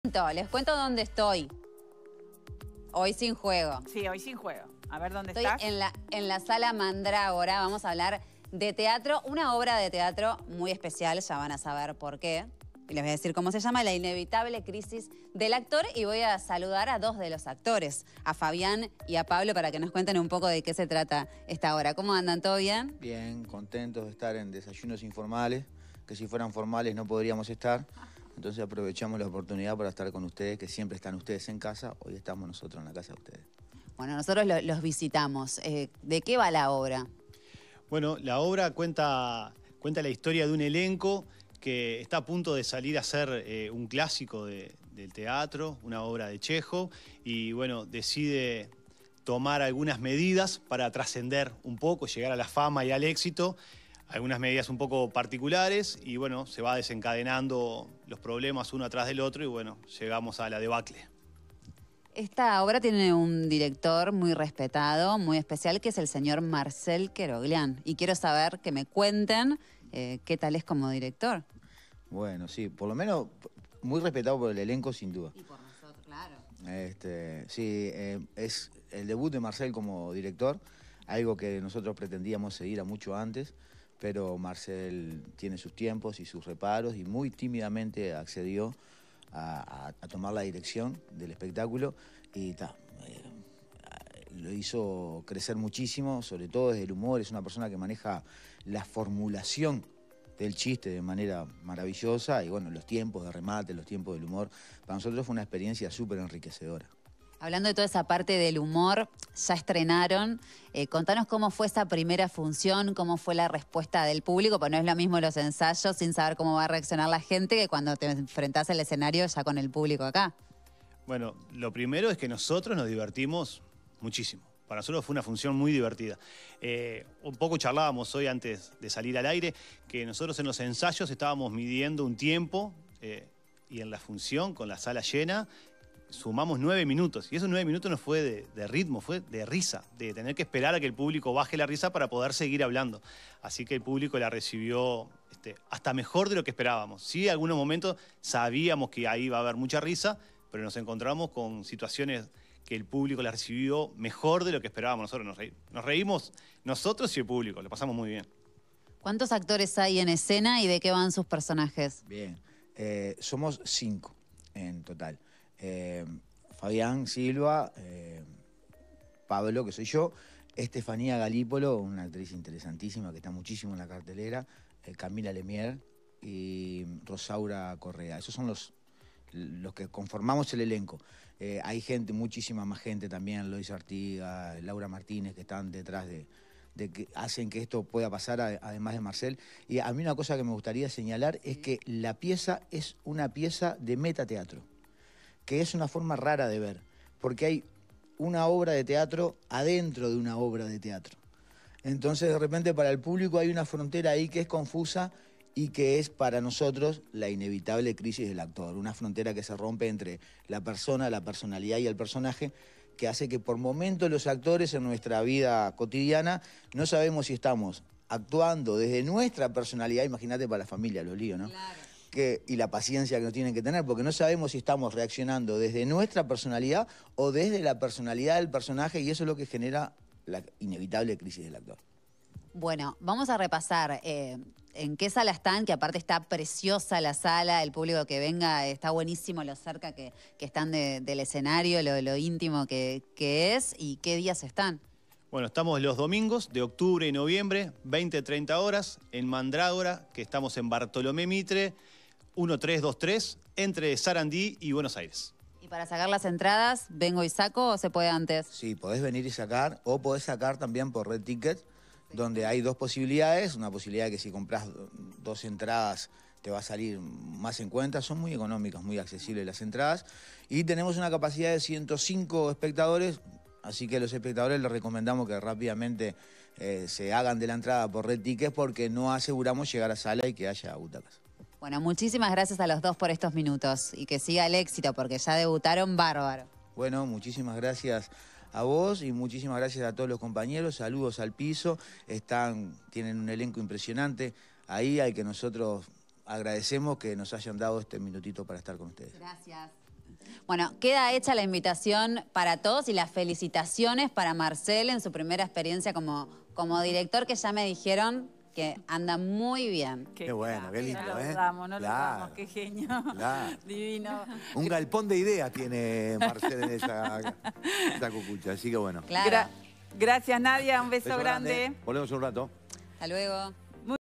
"La inevitable crisis del actor", una obra dirigida por Marcel Keoroglian
Tres actores en crisis llevan adelante un plan desopilante que saldrá mal